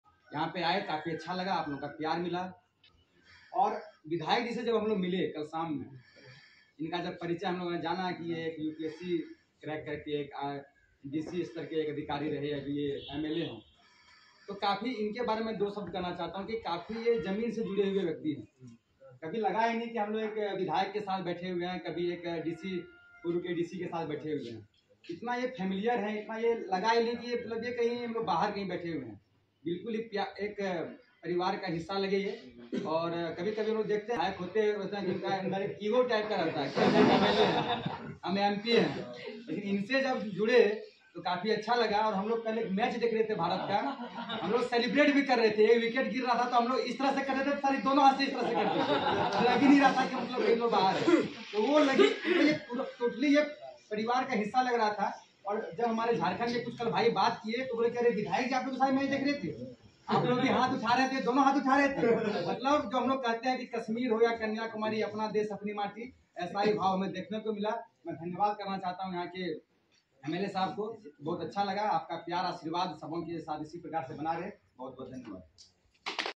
November 25, 2023
Subham Roy pursuing Bachelor of Education from Jharkhand, was the delegate in SEIL tour organised by ABVP, share his experience, the love and affection he received from DGangmei.

Subham Roy: I'm very fortunate to meet Hon'ble MLA Dinganglung Gangmei Retd. IAS In a world where power and ego flooded in the minds of some influencers, it is rare to find such a leader who stays grounded, and treats others with respect and kindness. I felt the love and care away from home and taught me a great lesson meeting him.

Though he possesses a great personality who crack UPSC, later MPSC, became a Deputy Commisioner and now an MLA in Manipur legislative Assembly. I cannot feel the distance between him and me.

It is indeed a remarkable moment to be the guest under his arms, regardless of his position or title, he cultivates humility and kindness. I see the  truth leadership in him he doesn't exert power or authority, but rather about serving others and making a positive impact in someone's lives especially to me on my tour to Khoupum Sub-Division, Noney District, Manipur.

 His humble attitude towards me and my team taught us that our purpose in life is to serve others rather than seeking personal glory.

I Wish him the very best in his career.